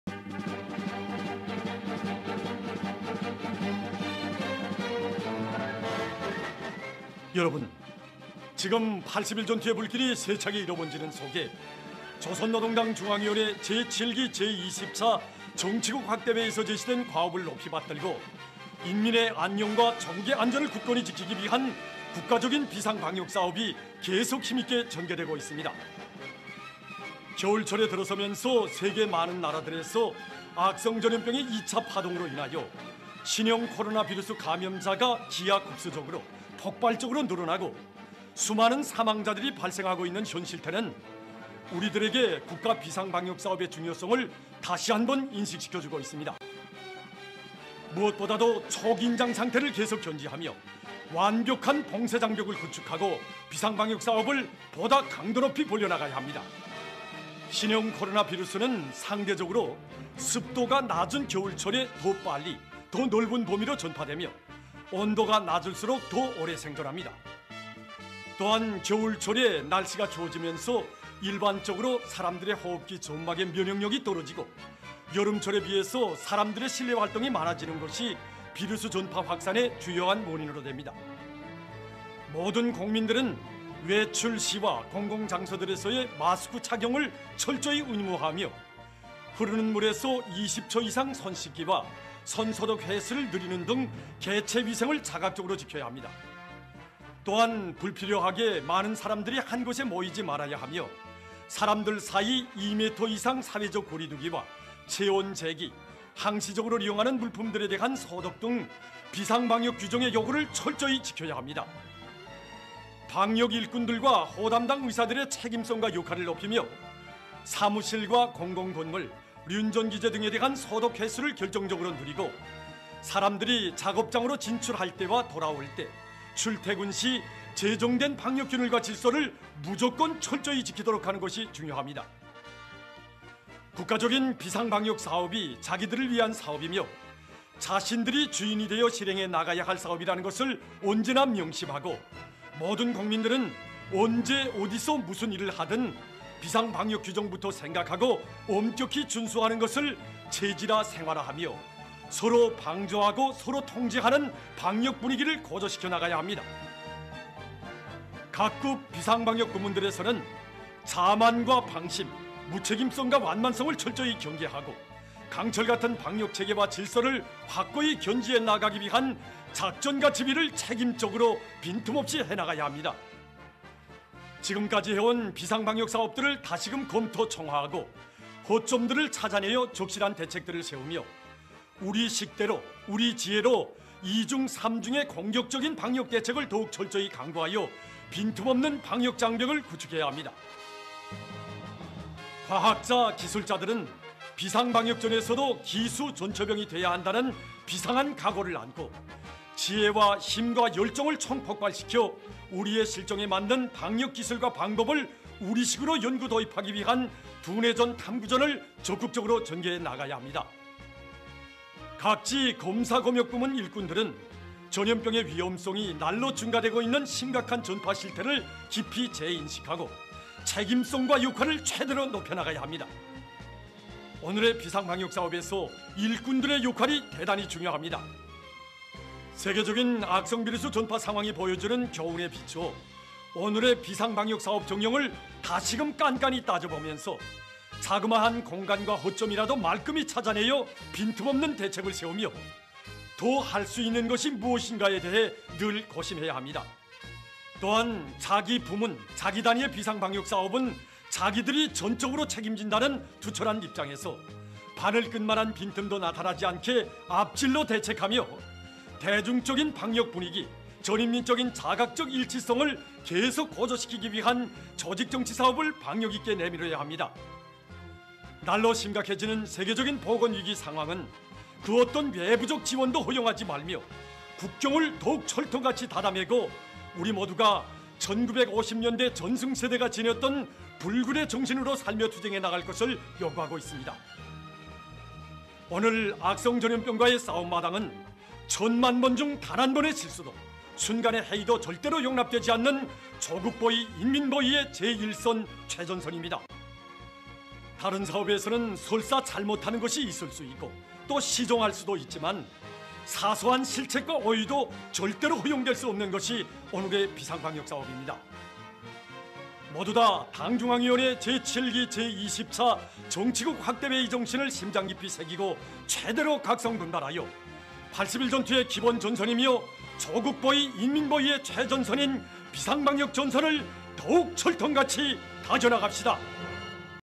북조선에선 코로나19 방역 지침 "이렇게 철통같이 완전 봉쇄한다!"
[박귀성 기자]=-조선민주주주의인민공화국 코로나19  방역 지침, 북한 보건당국이 북한 전역에 내린 코로나19(악성 비루스) 방역 지침을 북한 인터넷 선전선동매체들이 28일 일제히 조선중앙TV가 방송한 악성비루스 방역 지침 관련 보도 내용을 앞다투어 보도했다.